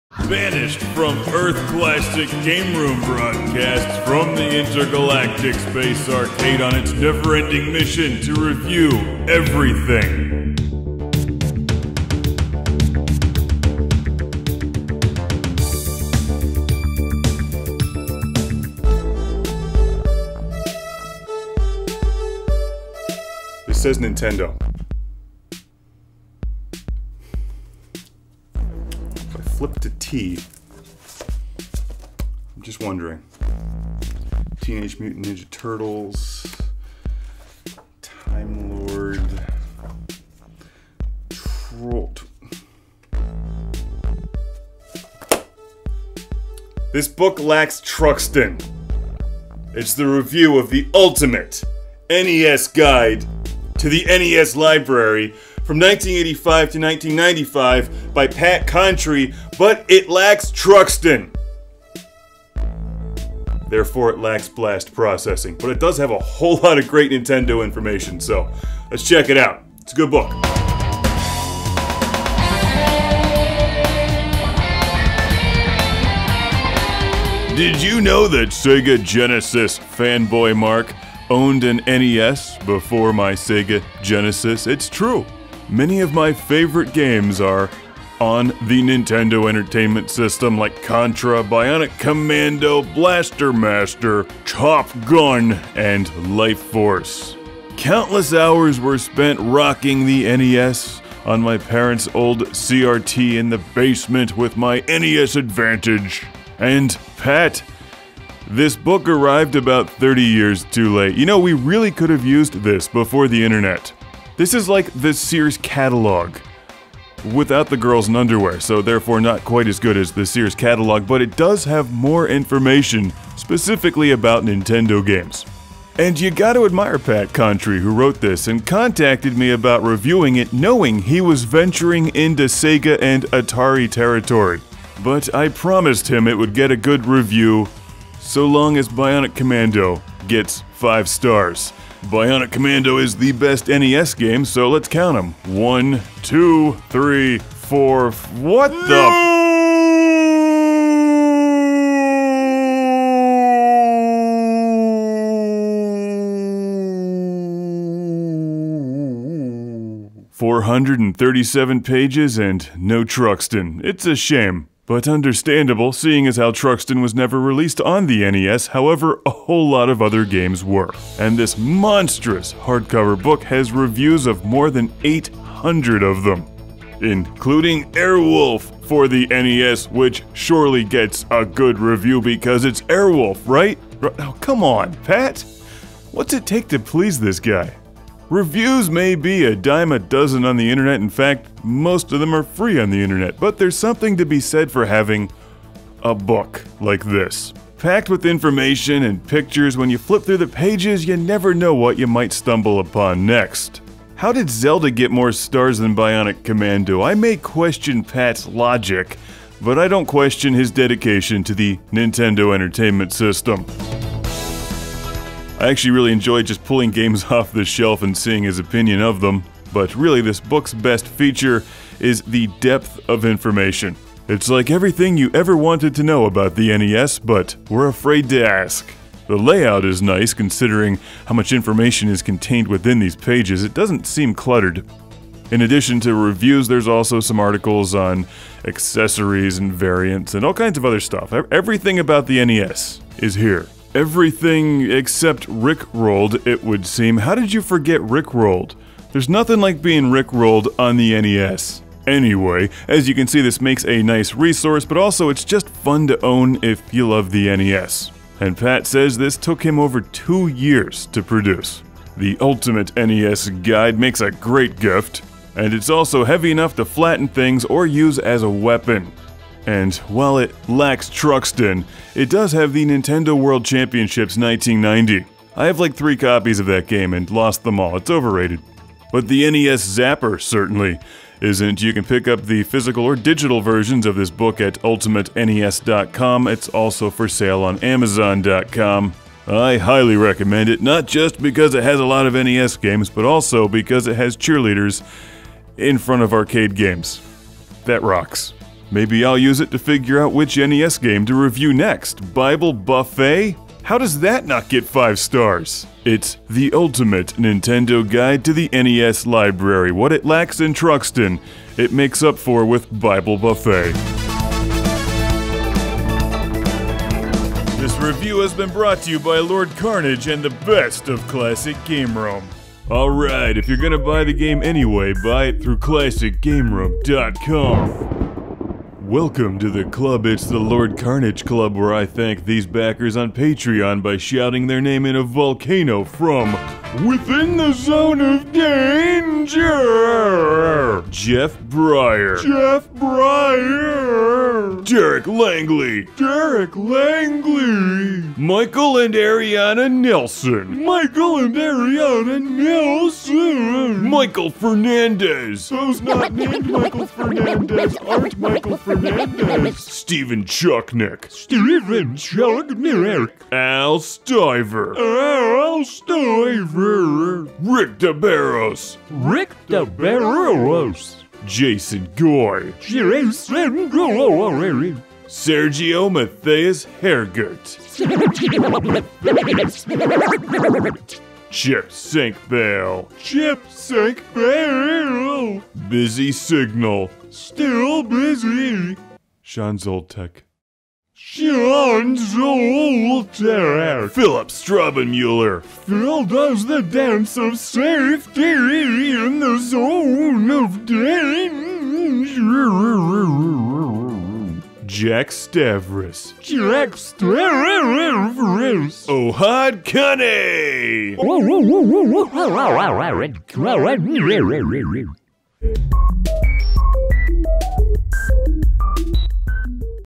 Classic Game Room - ULTIMATE NINTENDO GUIDE TO THE NES LIBRARY 1985-1995 review
- Ethel the Cyborg Ninja Comic Book Series
- Kawaii Ocean Clothing and Webtoon
- Omega Ronin Comic Book Series

Classic Game Room reviews the ULTIMATE NINTENDO GUIDE TO THE NES LIBRARY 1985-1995 by Pat Contri (Pat the NES Punk), a massive 450+ page book with over 800 NES video game reviews, information, photos, artwork and more!

CGR Ultimate Nintendo Guide to the NES Library YouTube review has video footage of this monster hardcover book showing hundreds of glorious NES games in its pages!

Sadly it does not feature reviews of MUSHA or Truxton... because the NES lacks Blast Processing.